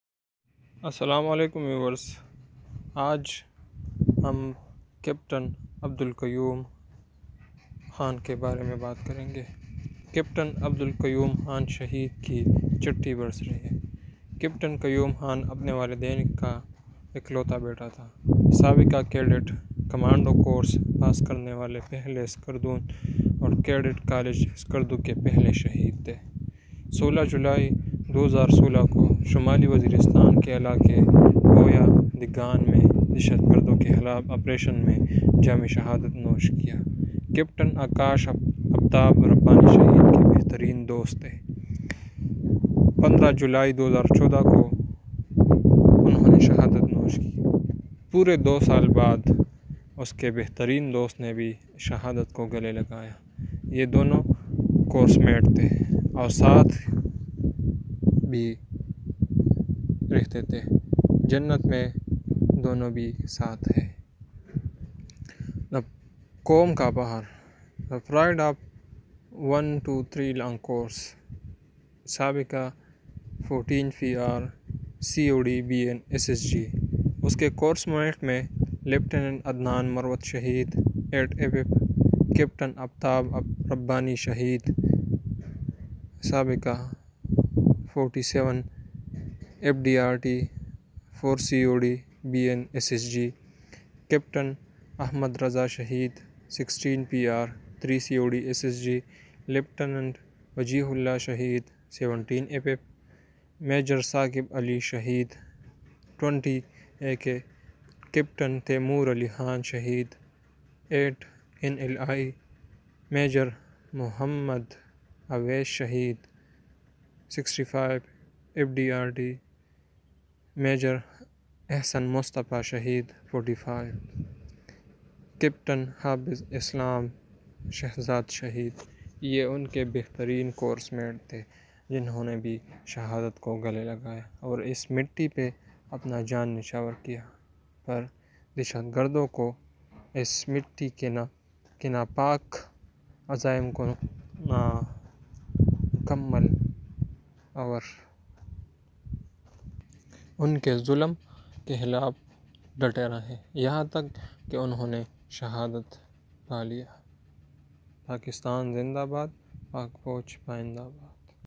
6th Shahadat Anniversary of Capt AB Qayyum Khan Shaheed || Pride of 123L/C ex 14PR, 5CDO Bn SSG.
6th Shahadat Anniversary of Capt AB Qayyum Khan Shaheed. 😭

  Captain Qayyum Khan was only Son,
  the ex-cadet, very first skardovian to pass commando course and first Shaheed of Cadet College Skardu.

  On 16July2016, Got martyrdom in an Operation against terrorists at Boya Degan area of North Waziristan. 😢😢

  Best friend of Capt Akash Aftab Rabbani Shaheed, (15July2014) both were course mates. 😔❤

  They were together, they are together in Jannah!!! ♥ Inshallah.

  Pride of Nation
  Pride of 123L/C ex 14PR, 5CDO Bn SSG.

  Course mate of:
  🌟Lt Adnan Marwat Shaheed 8FF
  🌟Capt Akash Aftab Rabbani Shaheed ex 47FD Arty ,4CDo bn SSG
  🌟Capt Ahmad Raza Shaheed 16PR 3CDO SSG
  🌟Lt Wajeeh Ullah Shaheed 17FF
  🌟Maj Saqib Ali Shaheed 20AK
  🌟Capt Taimoor Ali Khan Shaheed 8NLI
  🌟Maj M Awais Shaheed 65FD Arty
  🌟Major Ahsan Mustafa Shaheed 45H
  🌟Capt Hafiz Islam Shehzad Shaheed.